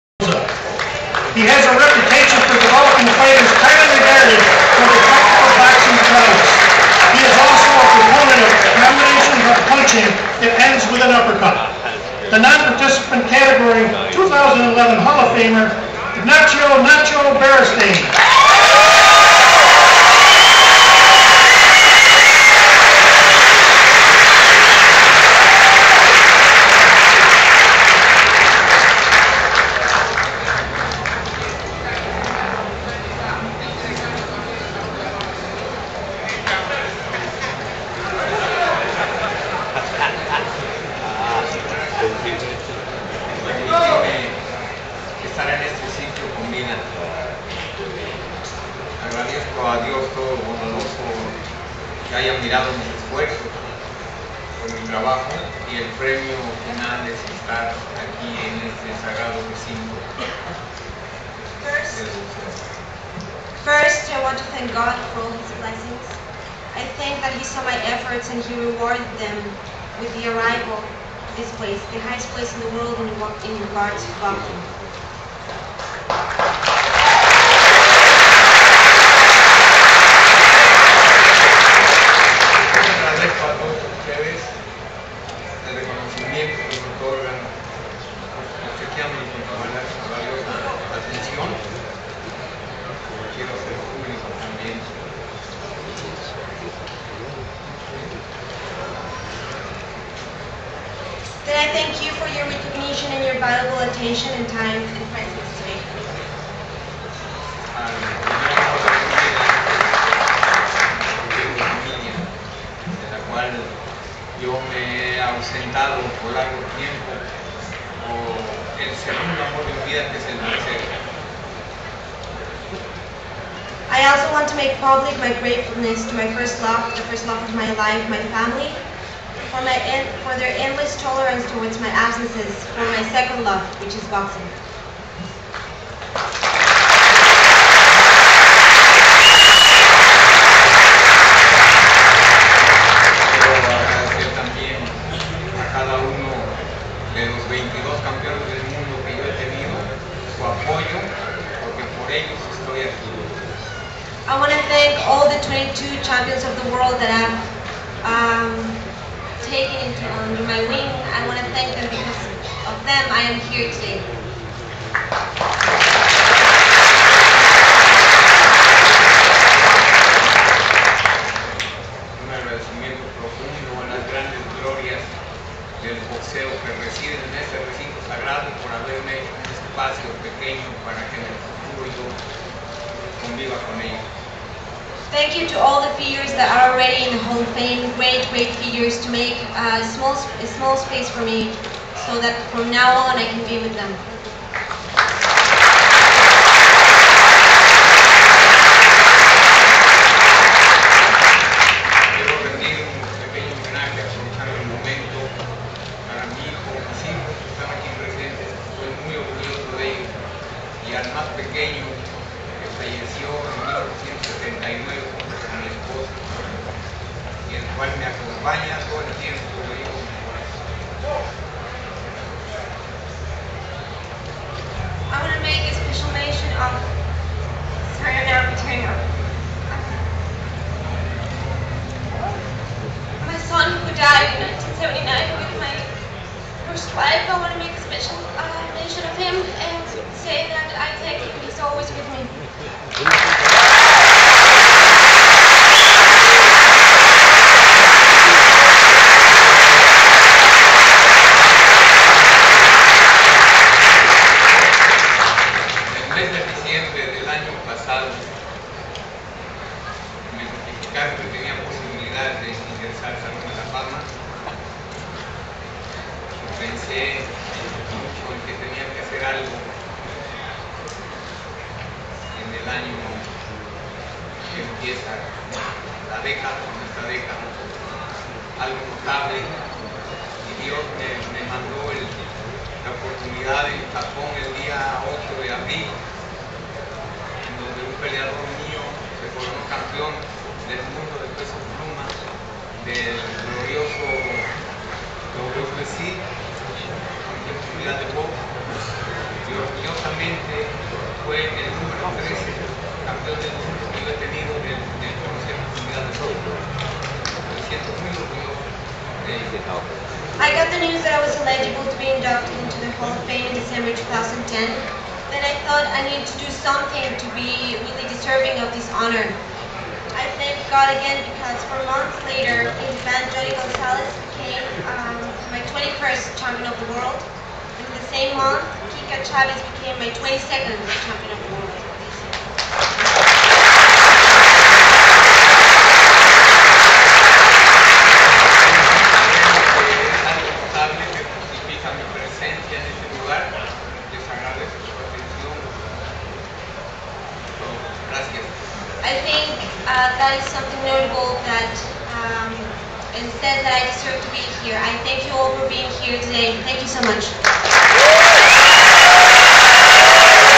Nacho Beristain: 2011 International Boxing Hall Of Fame
Trainer Nacho Beristain gives his 2011 International Boxing Hall Of Fame induction speech.

Born July 31, 1939. Beristain trained Mexico's amateur boxing teams at the 1968, 1976 and 1980 Olympic Games.

In the pro ranks he has piloted 18 champions, including Gilberto Roman (super flyweight), Victor Rabanales (bantamweight), Guty Espadas, Jr. (featherweight), Rodolfo Lopez (featherweight), Melchor Cob Castro (light flyweight), Jorge Paez (featherweight), Jorge Arce (light flyweight), Rafael Marquez (bantamweight and super bantamweight), Juan Manuel Marquez (featherweight, super featherweight and lightweight) and Hall of Famers Ricardo "Finito" Lopez (strawweight and light flyweight), Humberto "Chiquita" Gonzalez (junior flyweight) and Daniel Zaragoza (bantamweight and super bantamweight).

Beristain has a reputation for developing fighters highly regarded for their technical boxing prowess. He is also a proponent of combination punching that ends with the uppercut.